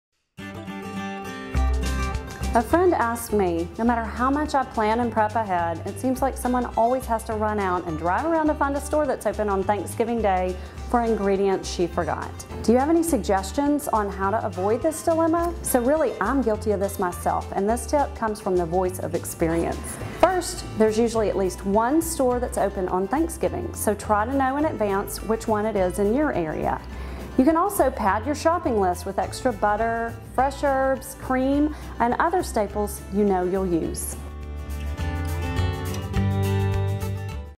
Holiday Hotline Tip: Avoid the Last Minute Supermarket Dash
Turkey Troubles? Contact the SL Holiday Hotline:
800/366-4712 ext. 66531
Twitter and Facebook

Don't waste time on Thanksgiving day looking for an open store! Follow our plan-ahead tips so that when the big day comes you can focus on cooking the feast. Plan your menu with our Thanksgiving meal planner